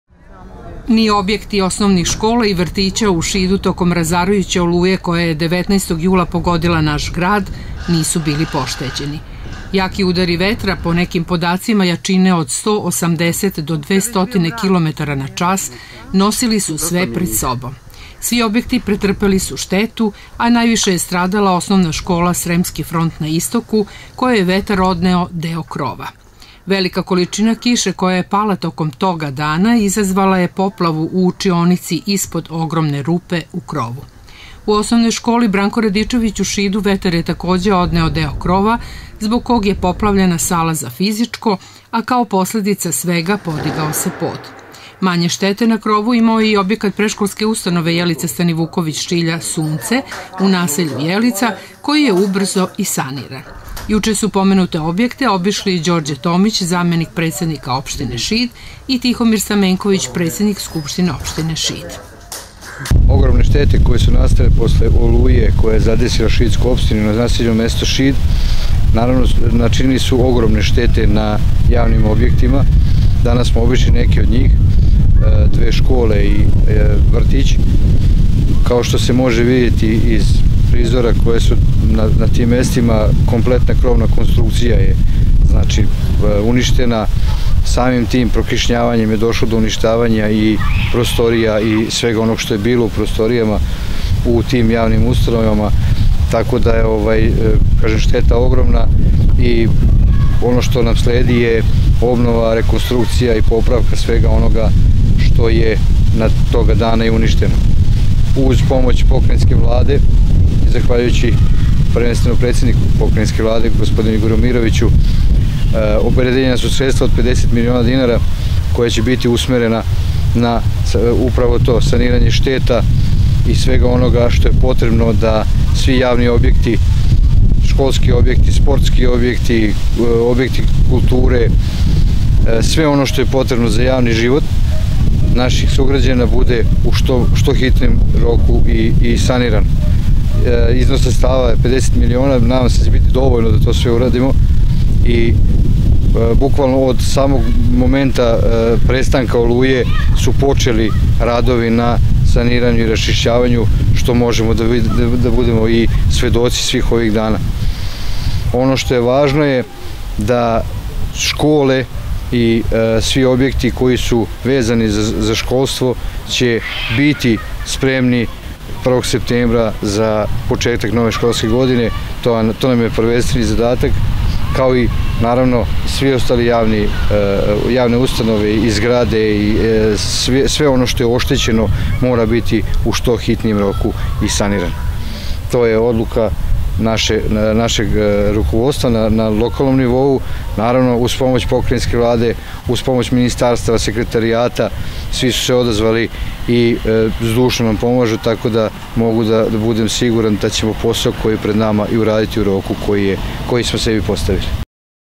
Obilazak oštećenih objekata škola i vrtića
Juče su zamenik predsednika Opštine Šid, Đorđe Tomić i predsednik Skupštine, Tihomir Stamenković obišli osnovne škole „Branko Radičević” i „Sremski front” i Predškolsku ustanovu „Jelica Stanivuković – Šilja” kako bi sagledali nivo oštećenja na objektima koja su nastala nakon razorne oluje koja je 19. jula pogodila Šid. U razgovoru sa direktorima škola najavili su hitnu rekonstrukciju ovih objekata iz sredstava koja je za Šid već obezbedila, Pokrajina Vojvodine u visini od 50 miliona dinara.